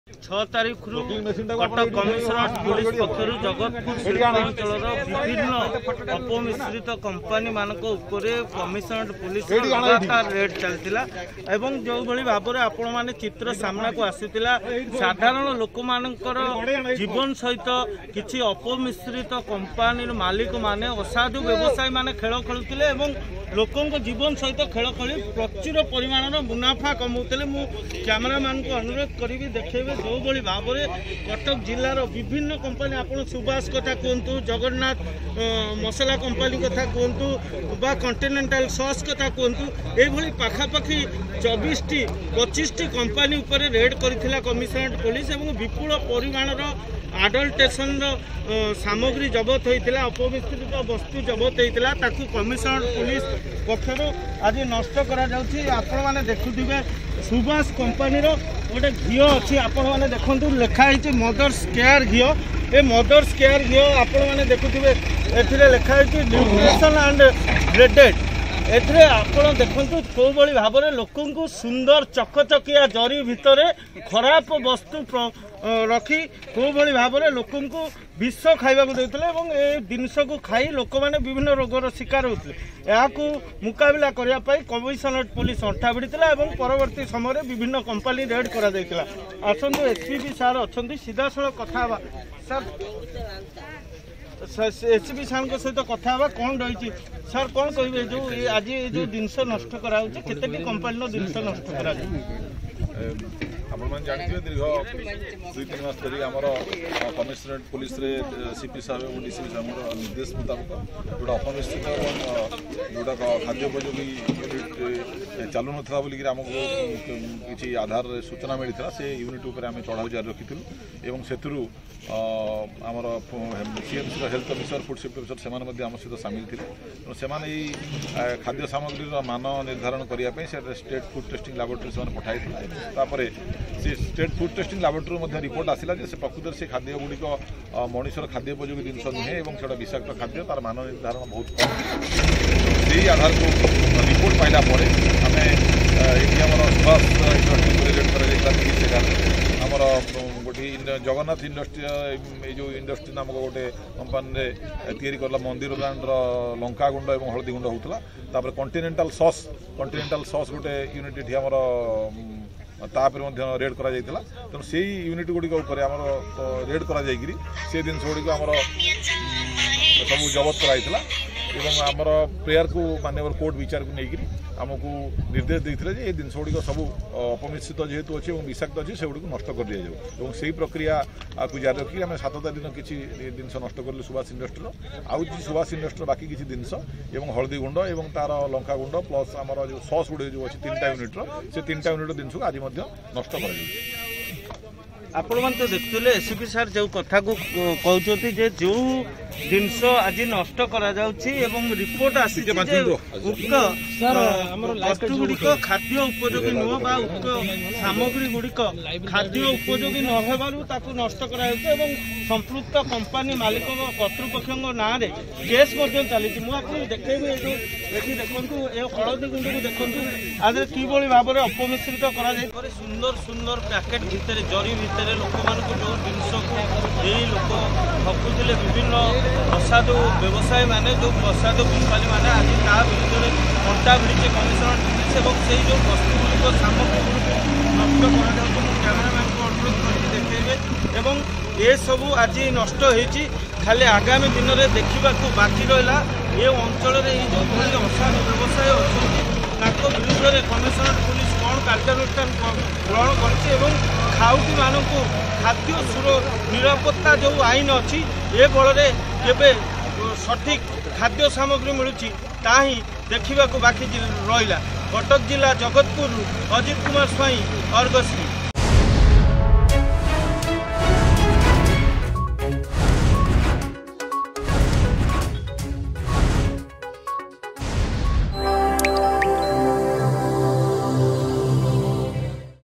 ଅପମିଶ୍ରଣକାରୀଙ୍କ ବିରୁଦ୍ଧରେ କମିସନରେଟ ପୁଲିସର କଡା କାର୍ଯ୍ୟାନୁଷ୍ଠାନ....
ଅପମିଶ୍ରଣକାରୀଙ୍କ ବିରୁଦ୍ଧରେ କମିସନରେଟ ପୁଲିସର କଡା କାର୍ଯ୍ୟାନୁଷ୍ଠାନ. ଜଗତପୁର ଶିଳ୍ପାଞ୍ଚଳରେ ନକଲି ଅପମିଶ୍ରିତ ଜବତ ସାମଗ୍ରୀକୁ ନଷ୍ଟ କରାଯାଇଛି.